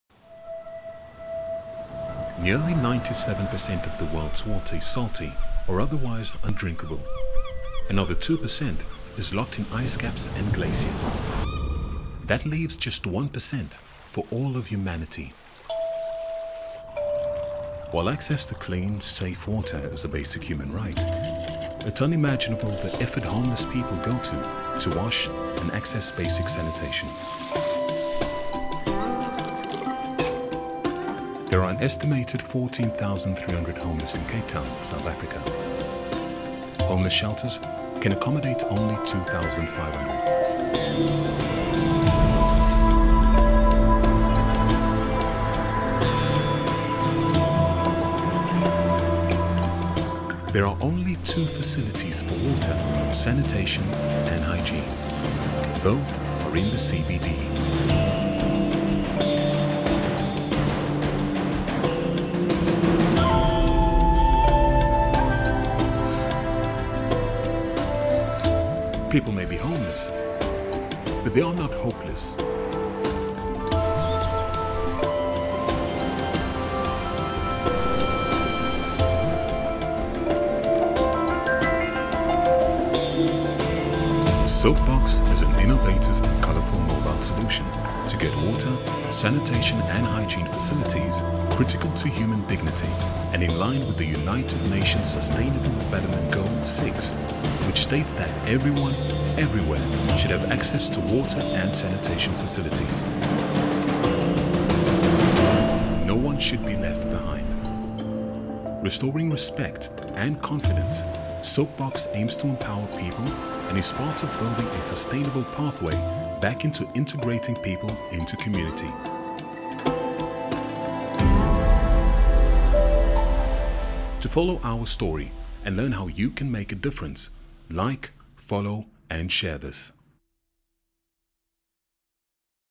BAZ-ART X VIVA CON AGUA: SOAP BOX
Water is a human right. Baz-Art team up with Viva Con Agua to create a wash facility for Cape Town's public living without homes.